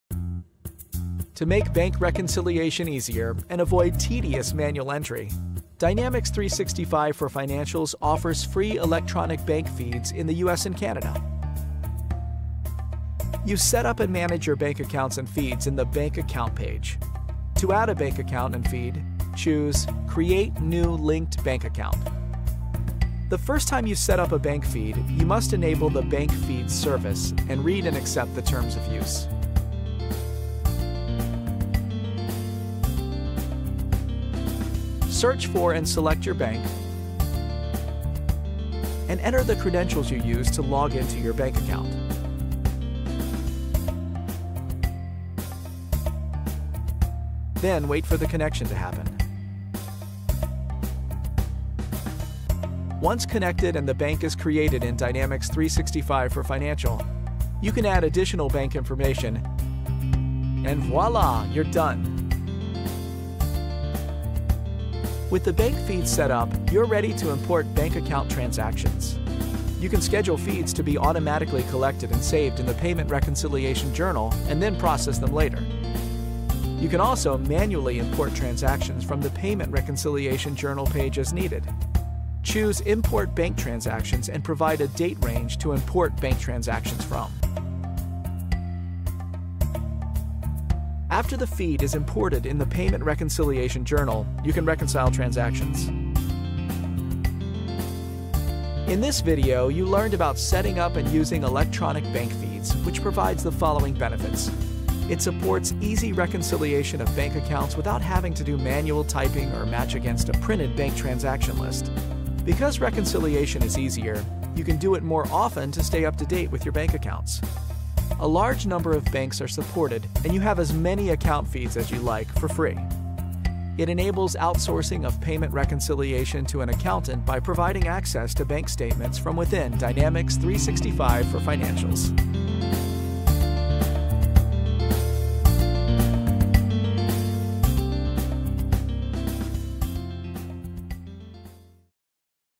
Set up bank integration in Microsoft Dynamics 365 for Financials
To make bank reconciliation easier and to avoid tedious manual entry Dynamics 365 for Financials offers free electronic bank feeds in the US and Canada.